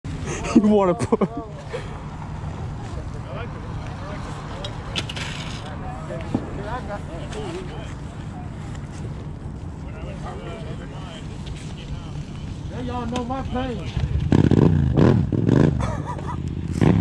636 getting bump started during wheelie wednesday | Seattle, WA
636 getting bump started during wheelie wednesday
Seattle, WA
Supermoto